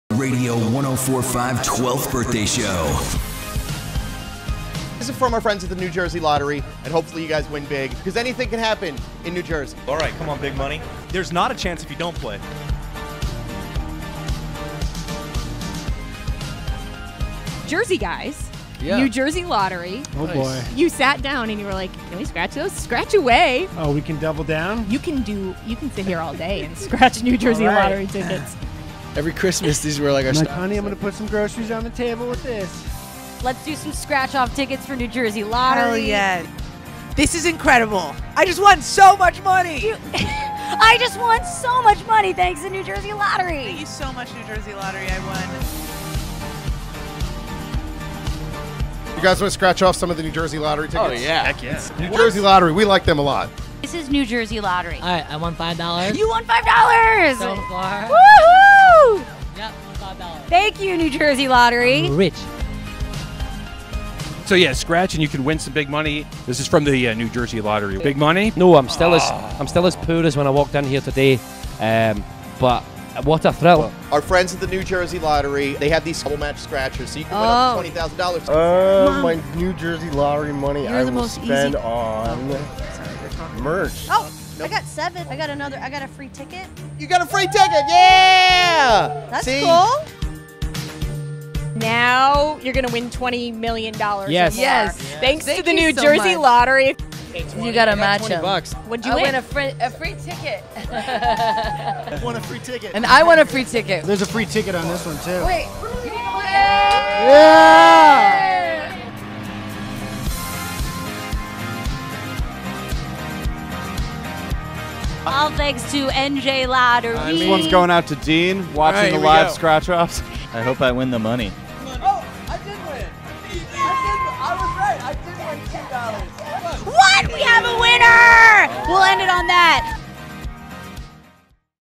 The Lumineers, Death Cab For Cutie, Grouplove, and more win with the New Jersey Lottery!
The Lumineers, Death Cab For Cutie, Grouplove, Phantogram, The Revivalists, The Interrupters, Lewis Capaldi, Des Rocs, and Weathers play the New Jersey Lottery scratch off tickets!